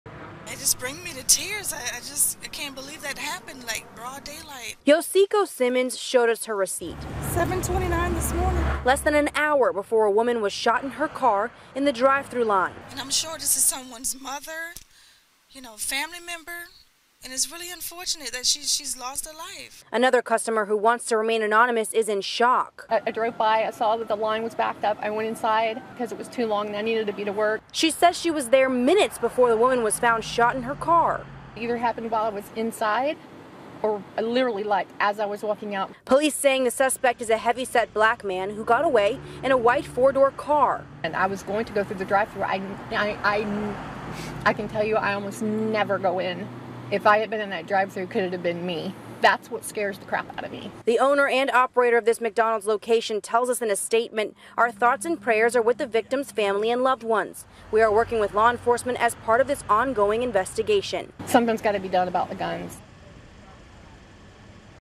Florida woman killed while ordering at McDonald's drive-thru
A Florida woman was ordering food at a McDonald's drive-thru when authorities say someone walked up and fatally shot her.